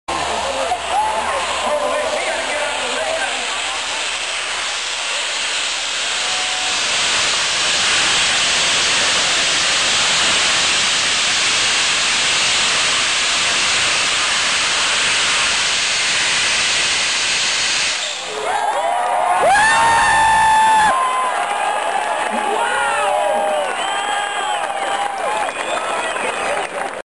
Jet Pack!
This dude from Go Fast blasts down the track